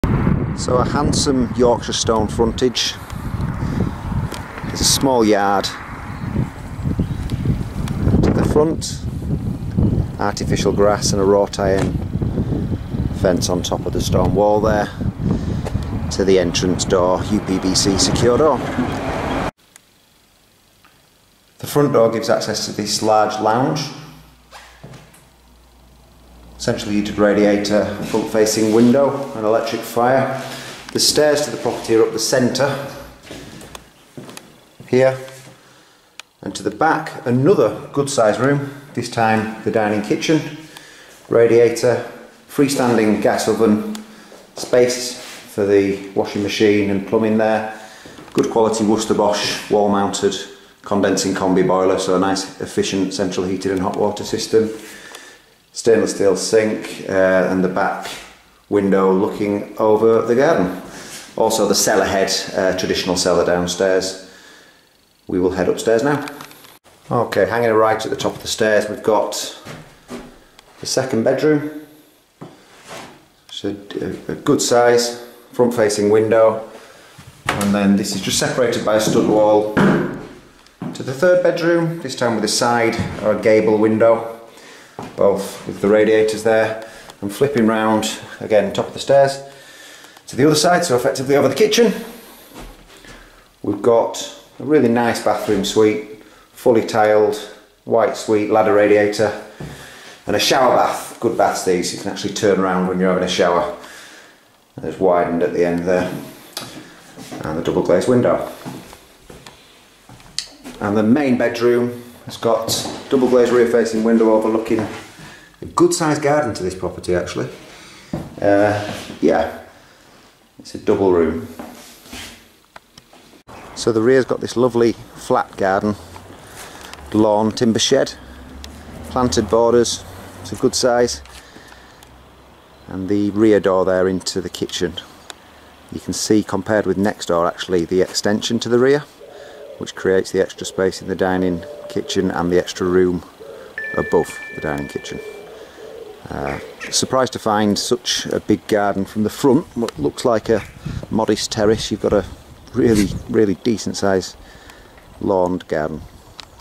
Greenside Road, Mirfield. Whitegates video viewing.
A walk around this attractive, stone fronted 3 bedroom semi-detached property.
Having a large lounge, good size dining kitchen, cellar, 3 bedrooms (2 double) and house bathroom.  Outside a small lawn to the front and a large enclosed rear garden offering off street parking.
Ideal for a couple or family, with schools and transport links nearby.
Offered with no onward chain.
Offered with no onward chain, making an ideal first home or investment property.